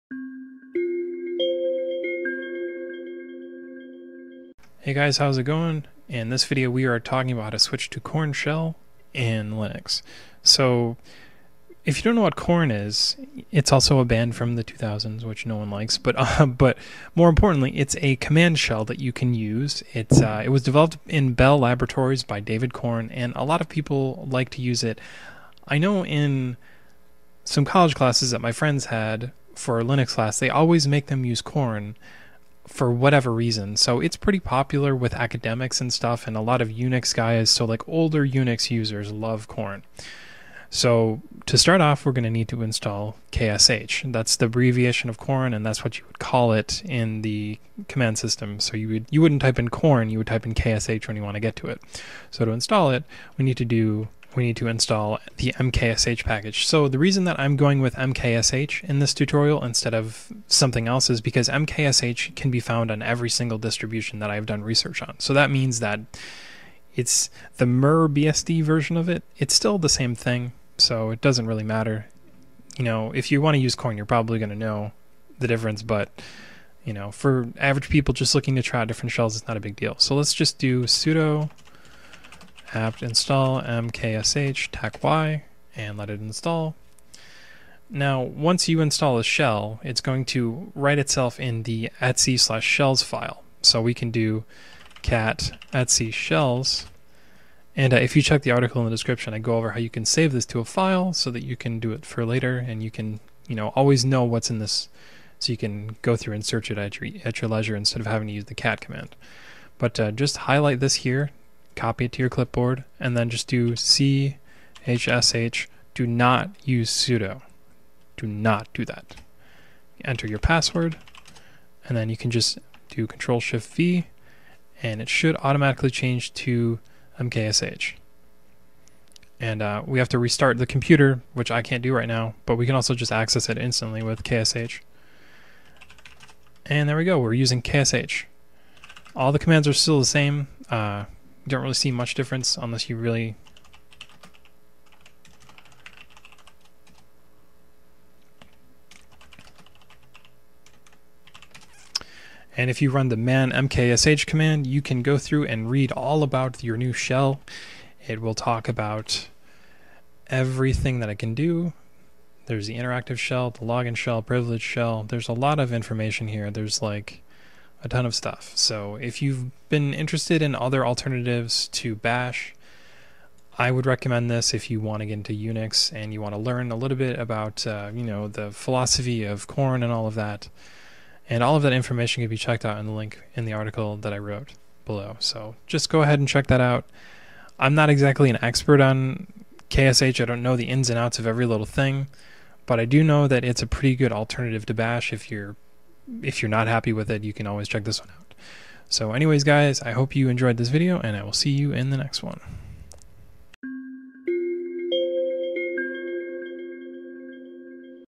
How to switch from Bash to Korn Shell on Linux
Korn Shell is a popular alternative to the Bash Shell that is commonly used as the default command-line system on Linux. The shell is backward compatible with Bash and borrows a lot of useful features found in C Shell.  Korn goes by Ksh for short and is very popular in the community. With a little know-how, it’s easy to get it working as the default shell on Linux! Follow along with our tutorial below and learn how!